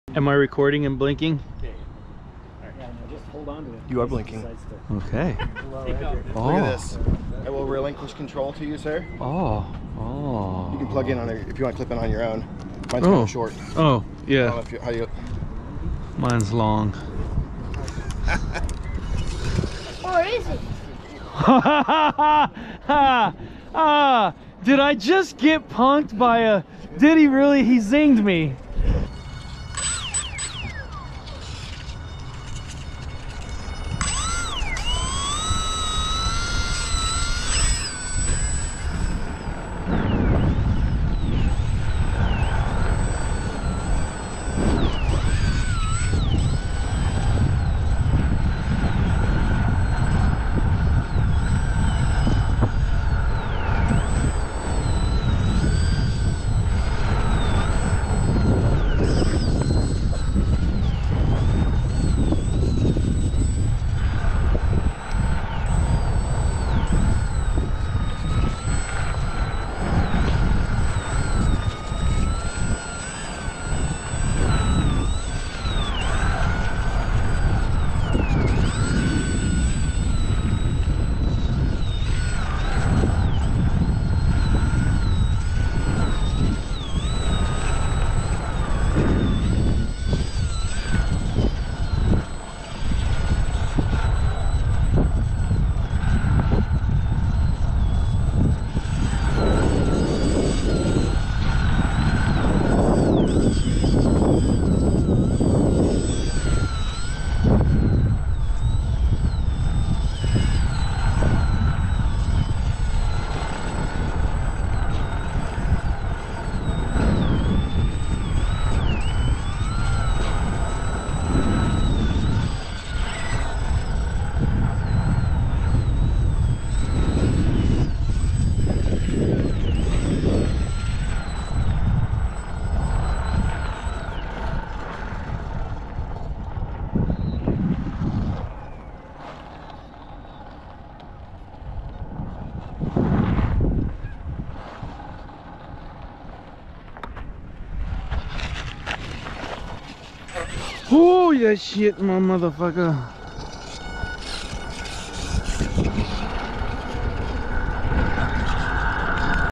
Can I Borrow Your Micro Draco?
My buddy, Daniel Zwijsen let me borrow his Micro Draco from eflite, a Horizon Hobby brand. This thing looks great, and flies great. Enjoy good unique footage!